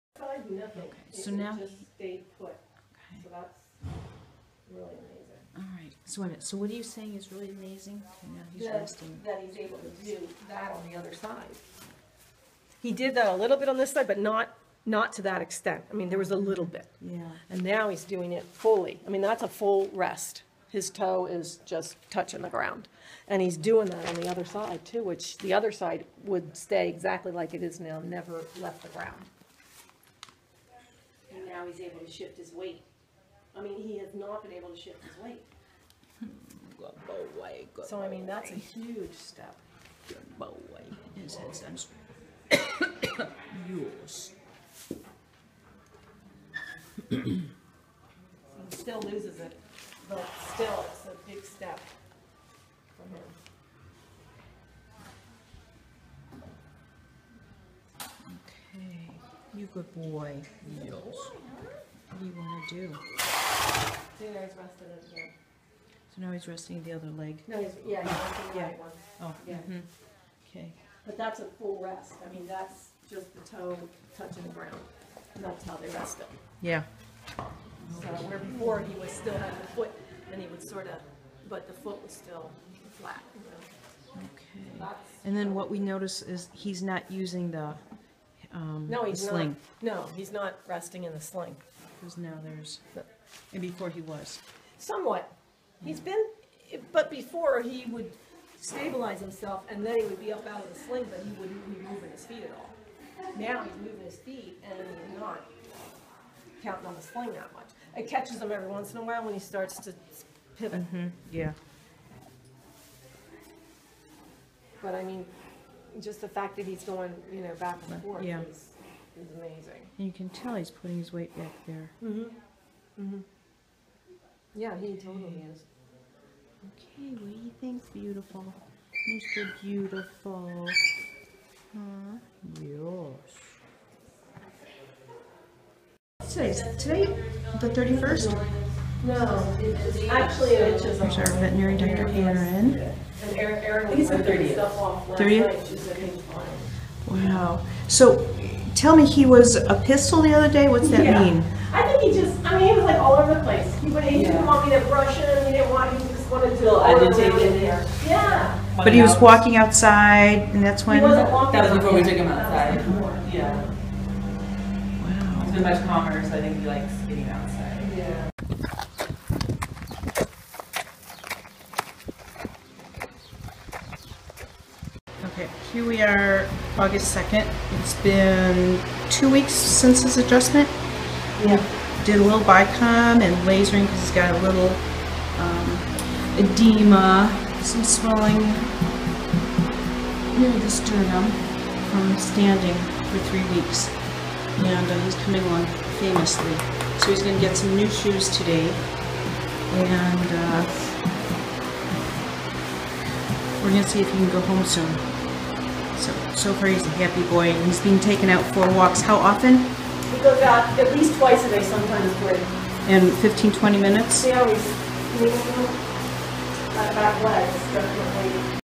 Fresian horse with flacid paralysis gets a chiropractic adjustment
This is a Prix St. George dressage competition horse that was found in pasture unable to get up. He had to be dragged by the legs via a chain to get him to a trailer and transport him to the vets at Alamo Pintado, where he was in a sling for three days unable to move his rear legs. That's when Dr. Gaber was called. She arrived to see his back legs crossed over each other and on tip toes. Watch to see his improvements after only one adjustment and after a week...he's walking again!